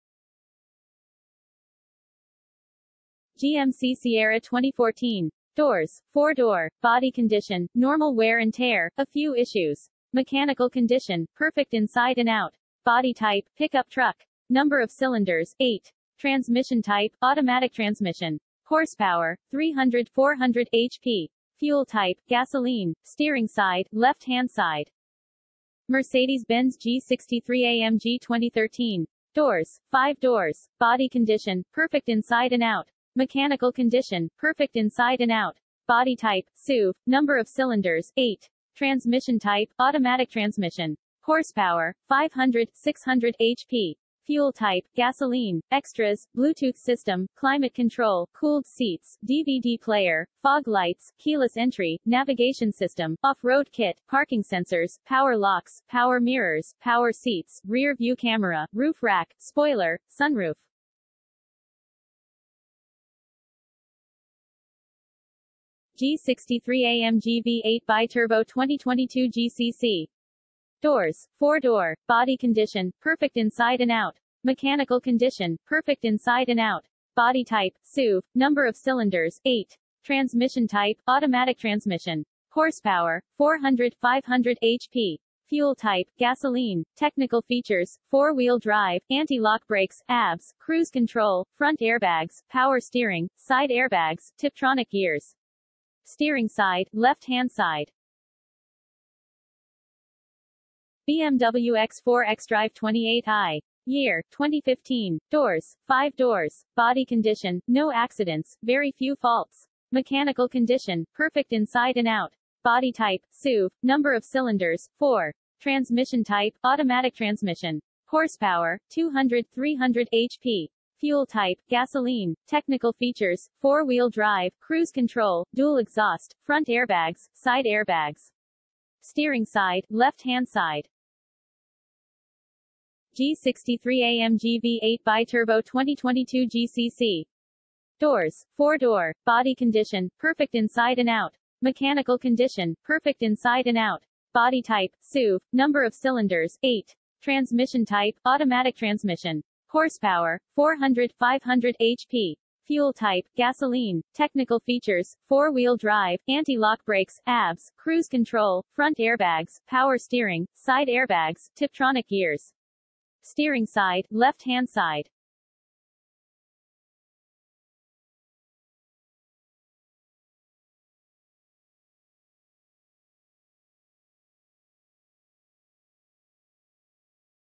New and used cars at reasonable prices 10/17/2022 - Car For Sale in Dubai
S 500 AMG GCC super clean car
AED 139,000
Souq Al Haraj, Sharjah, UAE

G63 AMG V8 Bi-Turbo 2022 GCC
AED 949,000
Ras Al Khor Industrial 3, Dubai, UAE

BMW x4 Xdrive28i
AED 89,000
Old Town, Dubai, UAE

MERCEDES-BENZ G-63 AMG 2013
AED 205,000
Al Awir, Dubai, UAE

GMC SIERRA 2014
AED 50,000
Al Khawaneej 1, Dubai, UAE